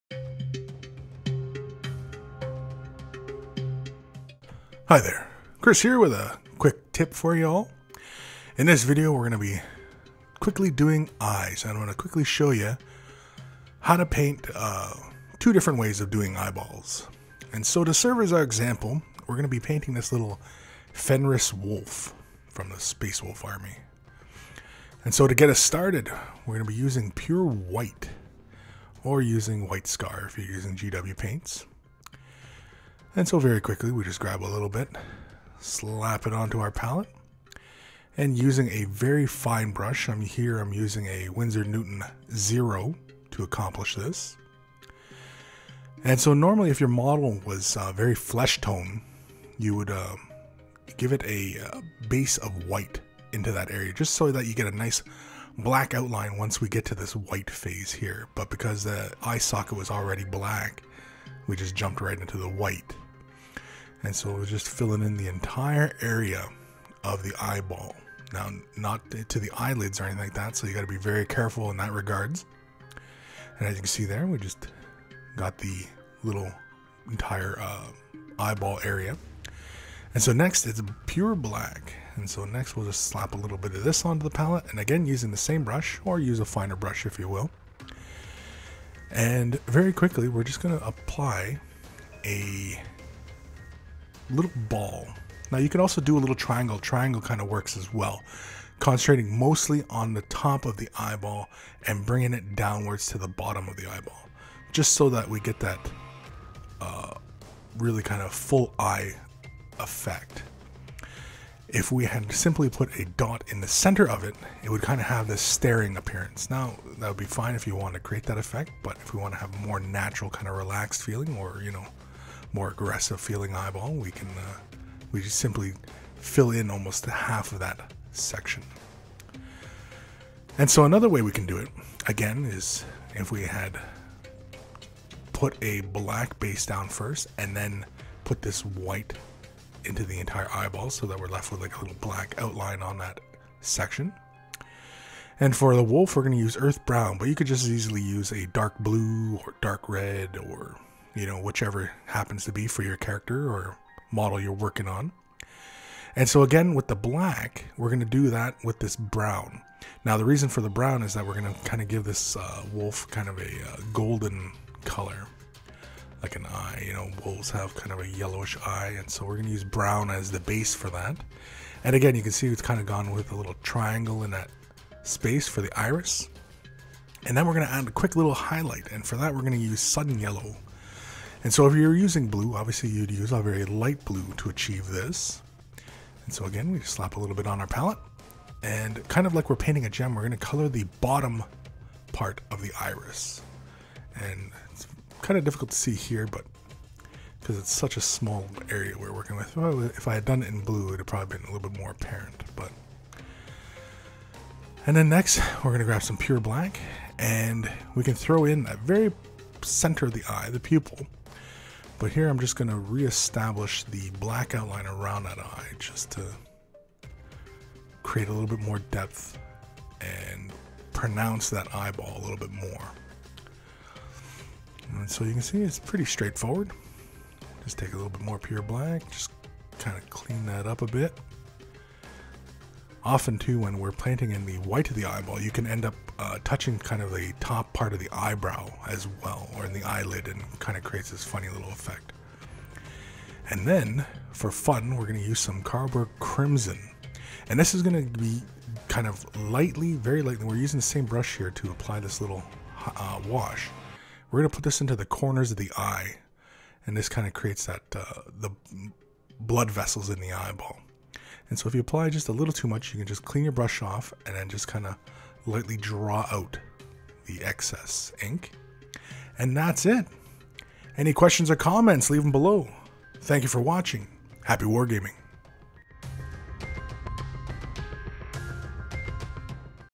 Quick Tip: Eyes
Kris demonstrates a couple of ways of painting eyes for your models.